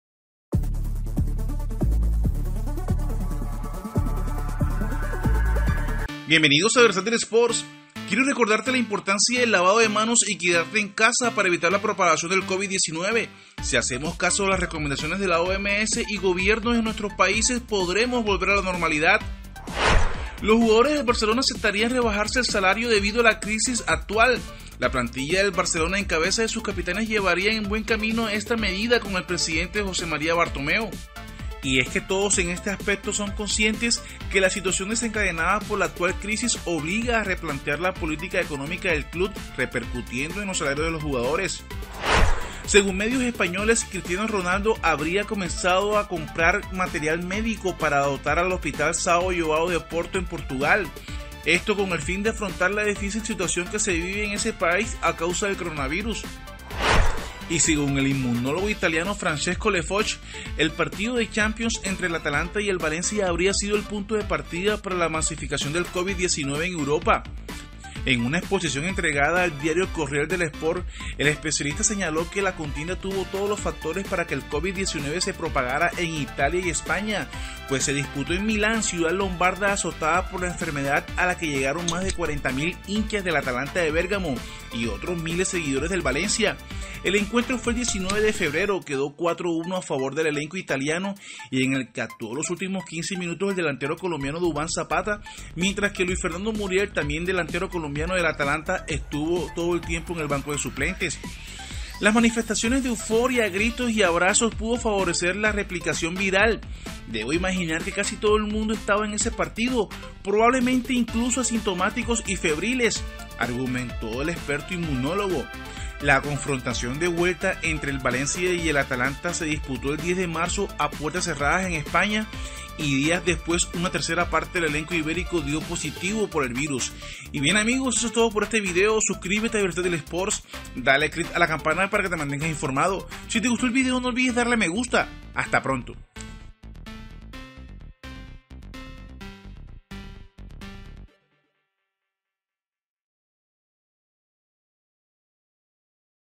El PARTIDO de FUTBOL que PROVOCÓ EXPANSION del CORONAVIRUS en EUROPA
EL PARTIDO de CHAMPIONS QUE PROVOCÓ EXPANSION del CORONAVIRUS ATALANTA vs VALENCIA